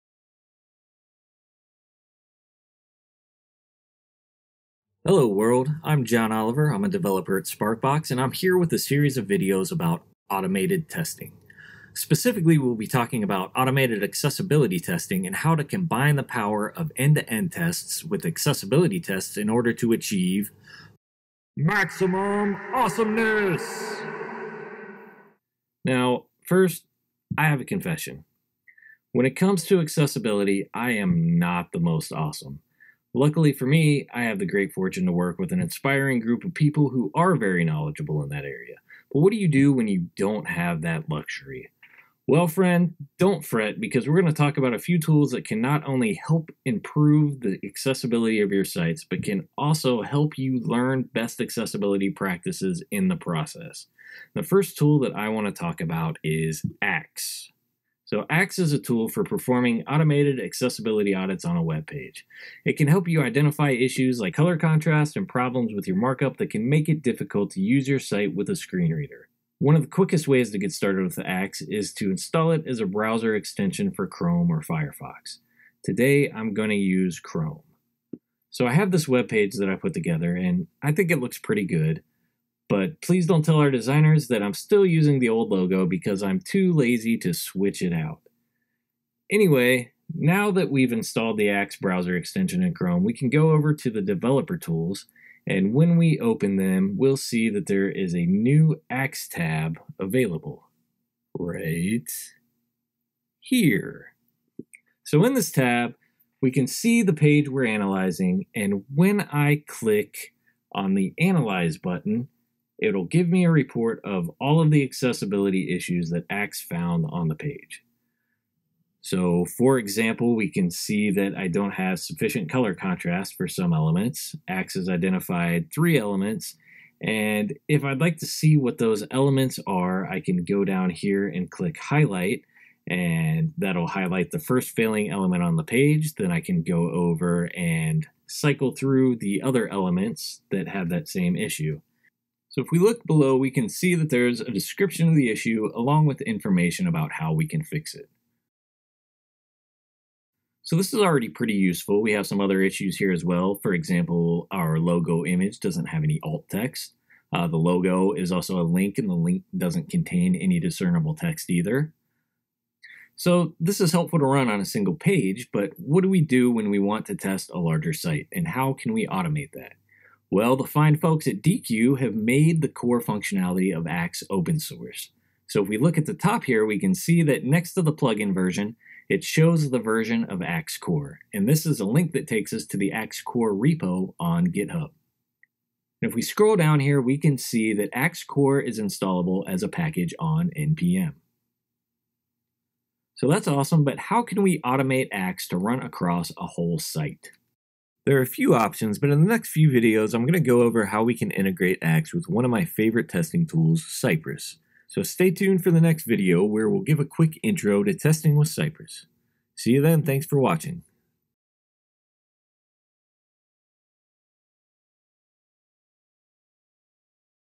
Accessibility Testing with Axe
Let's learn about Axe, a wonderful tool for helping us identify accessibility issues on a webpage. Axe is available as a browser extension to run directly on a webpage or as an npm package to run programmatically.